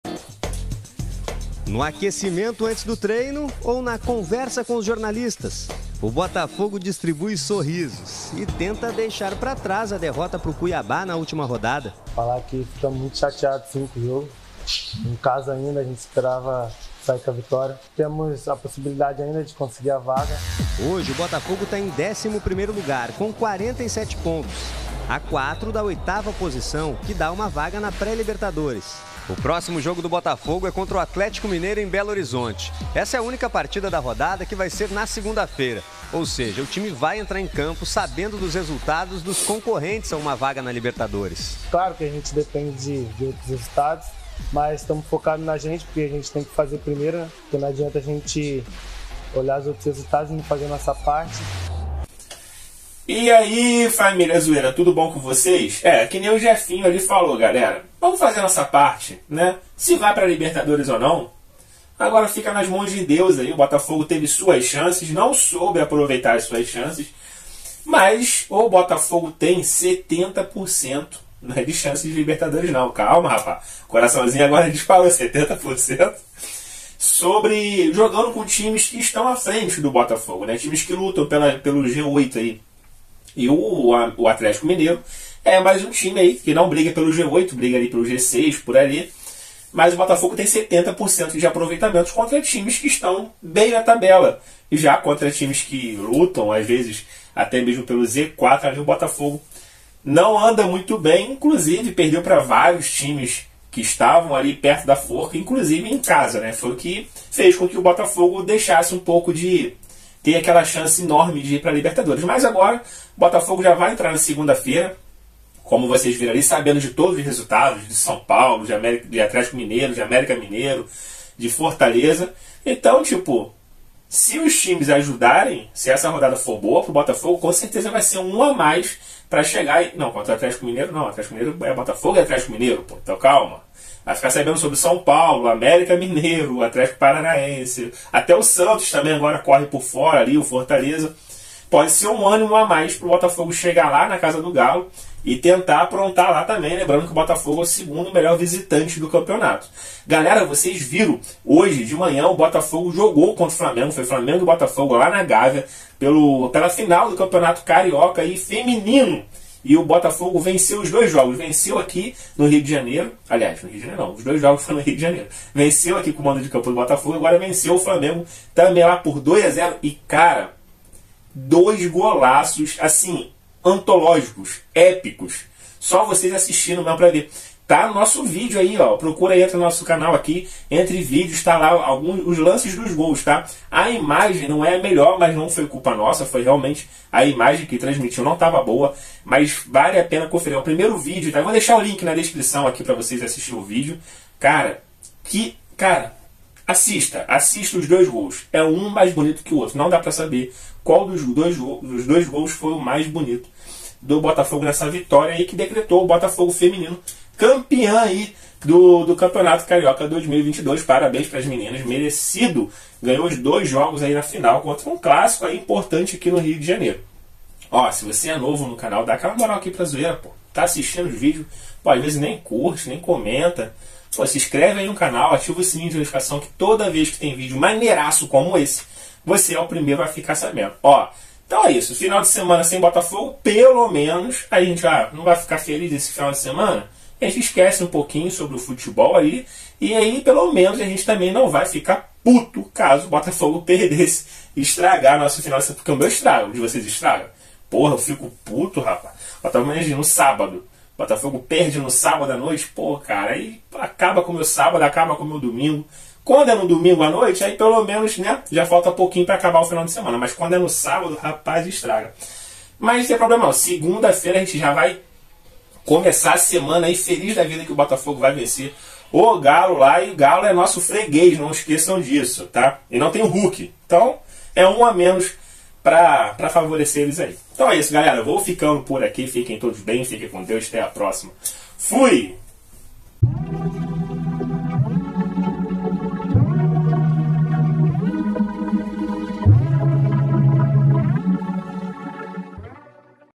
NOTÍCIAS DO BOTAFOGO HOJE..
BOTAFOGO SÓ JOGA NA SEGUNDA FEIRA MAIS TEM QUE FICAR DE OLHOS EM SEUS ADVERSÁRIOS HOJE E AMANHÃ..